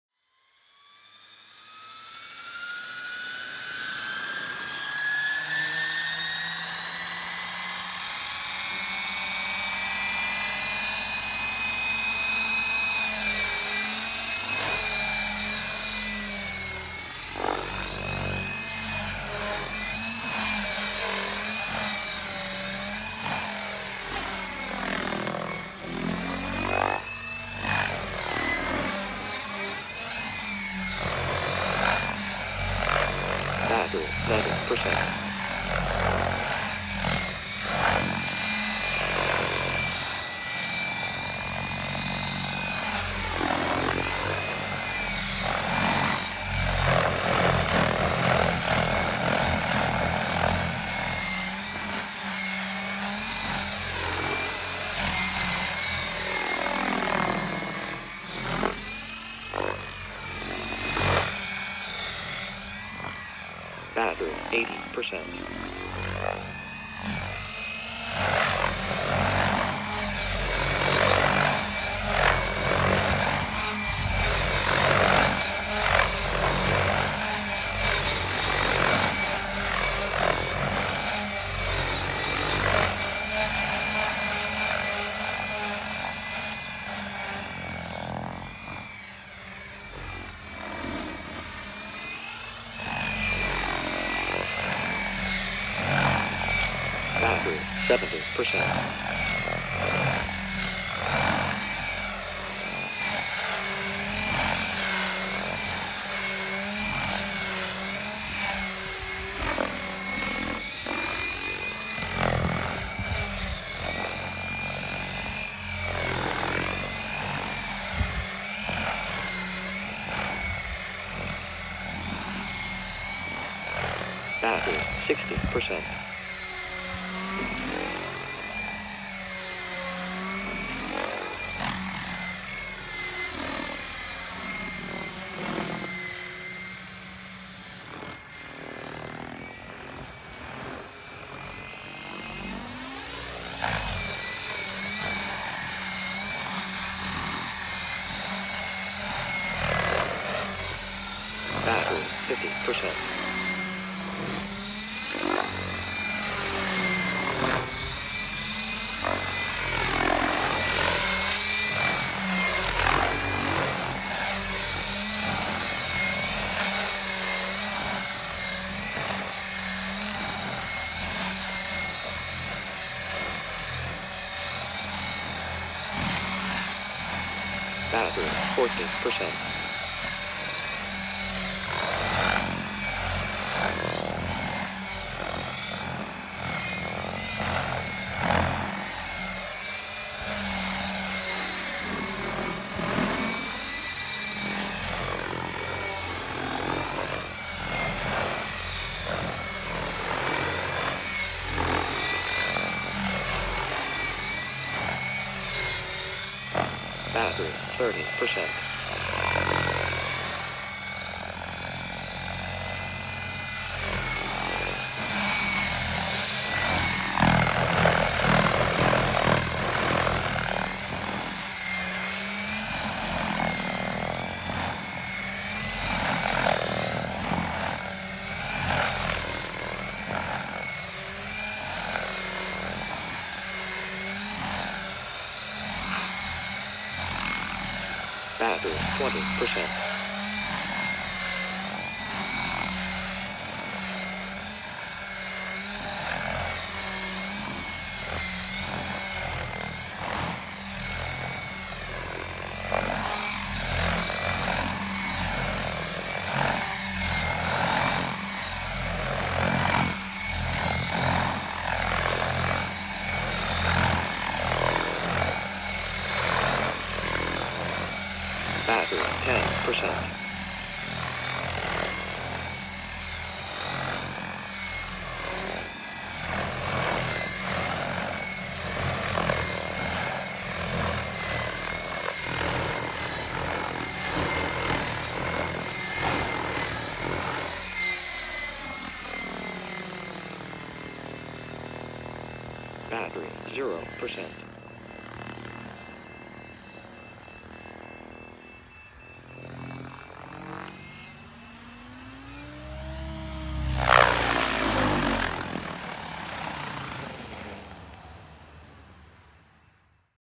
LOGO 600 SX | RT-610U | 6S Low RPM
Relaxed flight with my 600SX on 6S, which works really well because of my horribly overgeared drive train.

Thank you Dennis for shooting this video!